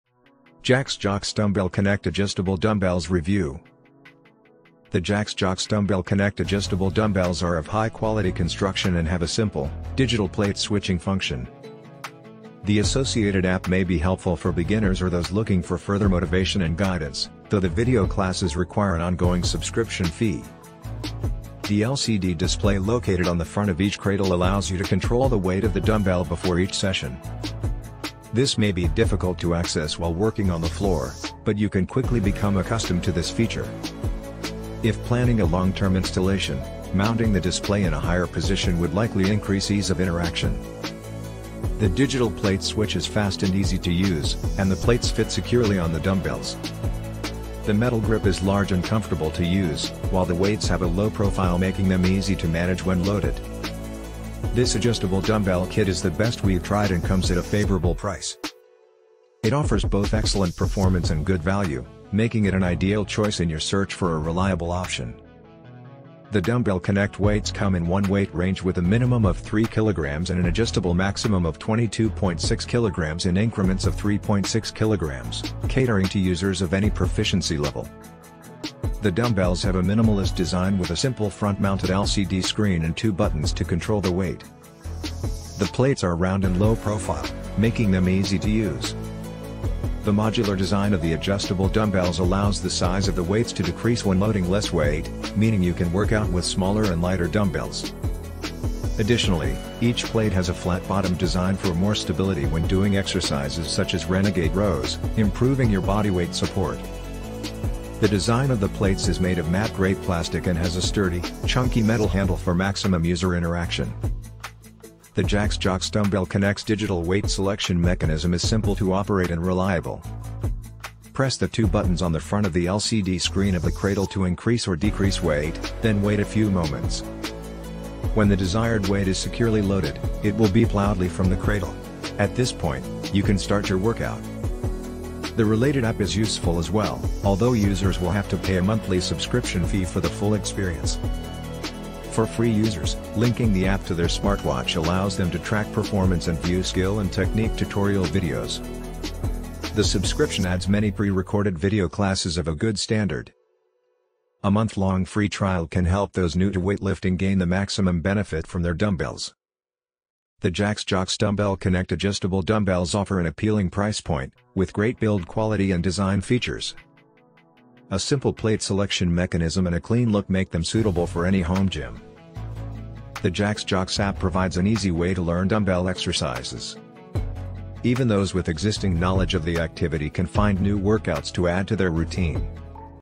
JaxJox DumbbellConnect Adjustable Dumbbells Review | Excellent Value for Money
In this video, we'll be taking a close look at the JaxJox DumbbellConnect, a set of adjustable dumbbells that allow you to customize your workout and save space in your home gym. We'll explore its features, ease of use, performance, and overall value, to help you decide if it's the right dumbbell set for your fitness routine. We'll also compare it to other adjustable dumbbells on the market and discuss some of its pros and cons. So if you're in the market for a new set of dumbbells, or just curious about the JaxJox DumbbellConnect, this review is for you!